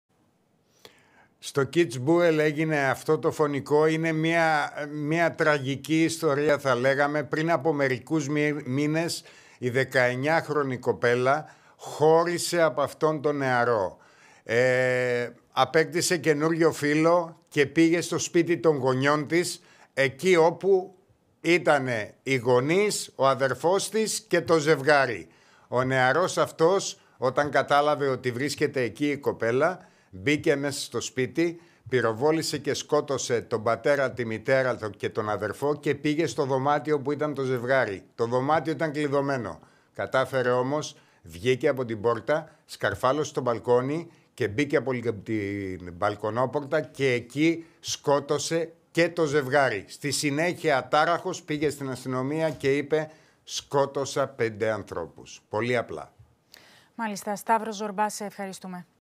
Σοκ στην Αυστρία: 25χρονος σκότωσε την οικογένεια της πρώην συντρόφου του
Ένας 25χρονος στο Κίτσμπουελ της Αυστρίας σκότωσε την 19χρονη πρώην σύντροφό του, τον φίλο της, τον αδελφό και τους γονείς της και στη συνέχεια παραδόθηκε στις αρχές, ανακοίνωσε σήμερα η αστυνομία.

Ο δράστης εισέβαλε στο σπίτι της οικογένειας γύρω στις τέσσερις το πρωί και σκότωσε τον πατέρα, την μητέρα και τον αδελφό της πρώην φίλης του, με την οποία είχε χωρίσει πριν από δύο μήνες.

Η ίδια με τον σύντροφό της κοιμούνταν σε ένα δωμάτιο που ήταν κλειδωμένο και έτσι ο δράστης βγήκε από το σπίτι, σκαρφάλωσε στο μπαλκόνι και μπήκε από το παράθυρο και τους σκότωσε.

Στη συνέχεια παραδόθηκε στο πλησιέστερο αστυνομικό τμήμα και είπε στους αστυνομικούς «σκότωσα πέντε ανθρώπους».